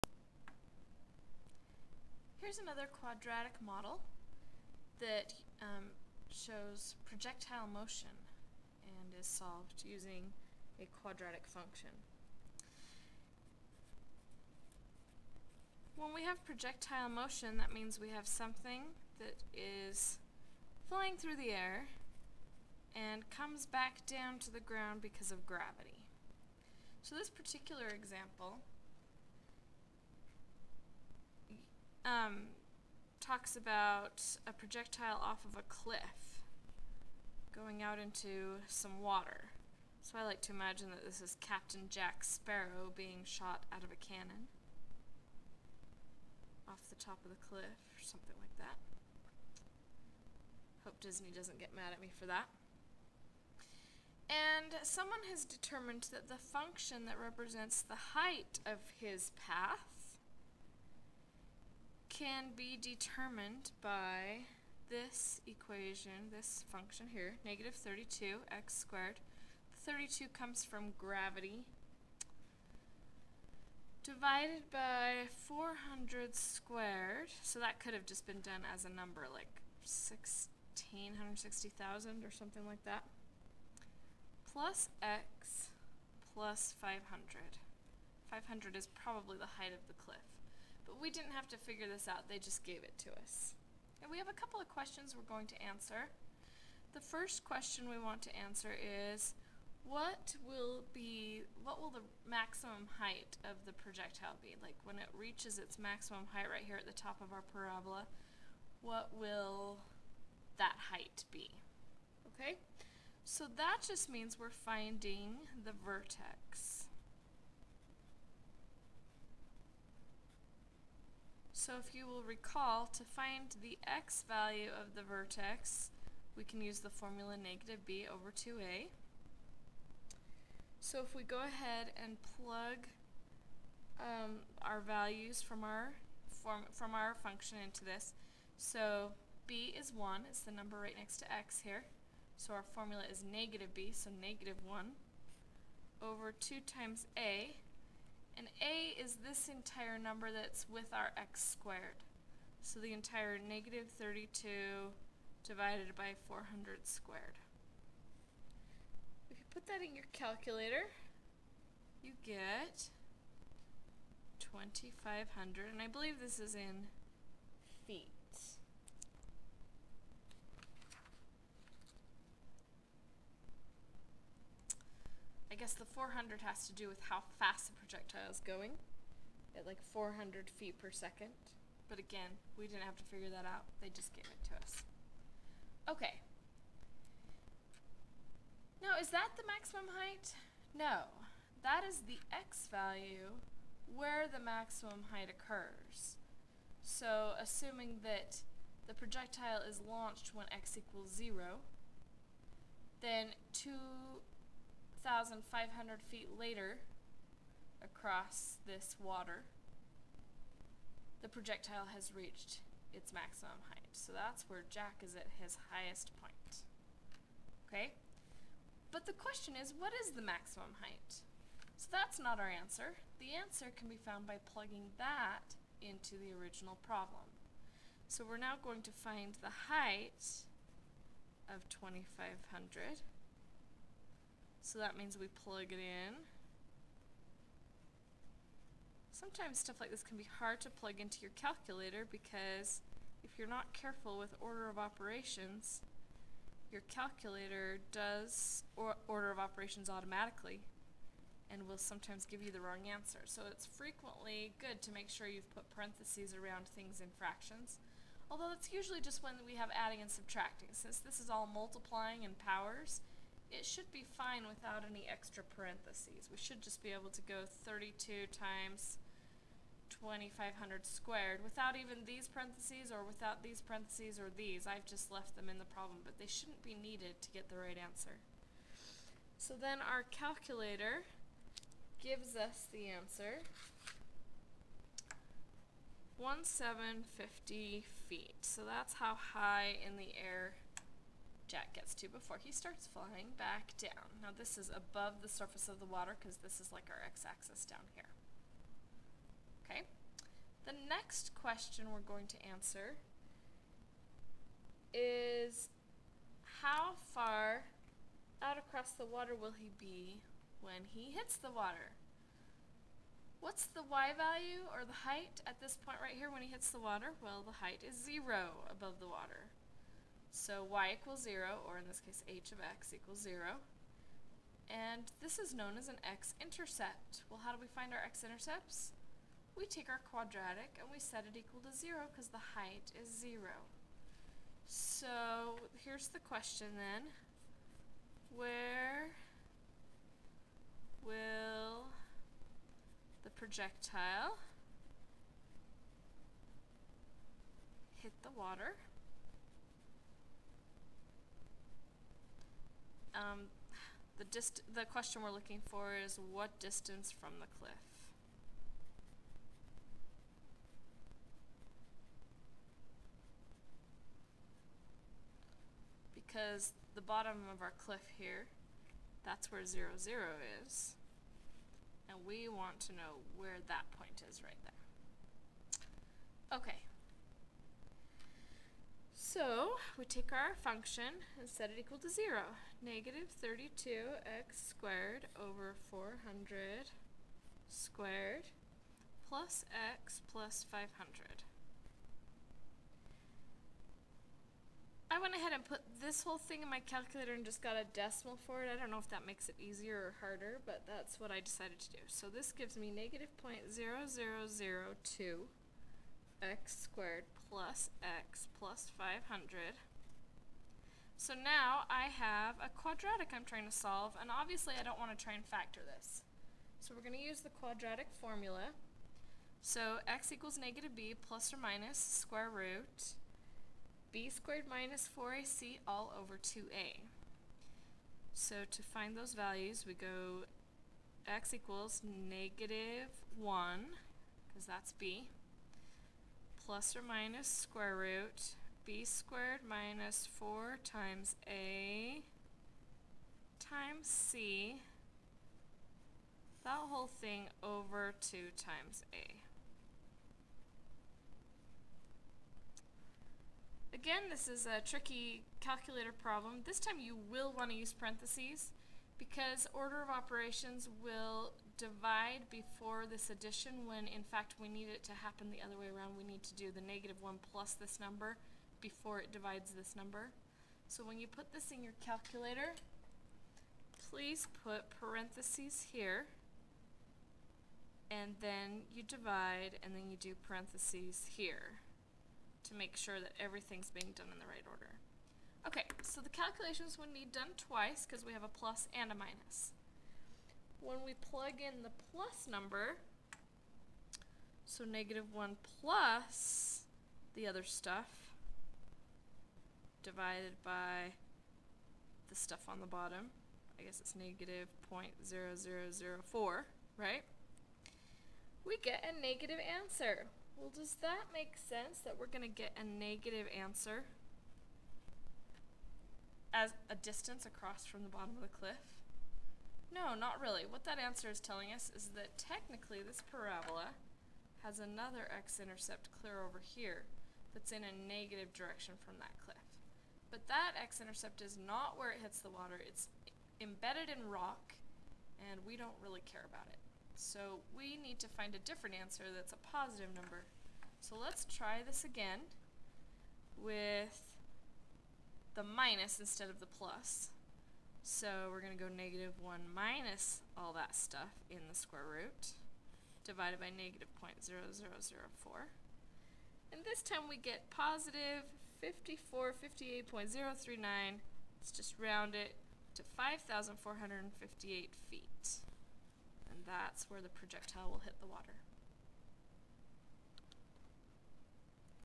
Quadratic Models: Projectile Motion
Given the function for the height of a projectile at a distance x from a cliff, we use properties of quadratics, including finding the vertex and using the quadratic formula to find the intercepts to answer questions like: What will be the maximum height of the projectile? At what distance from the cliff with the projectile hit the water?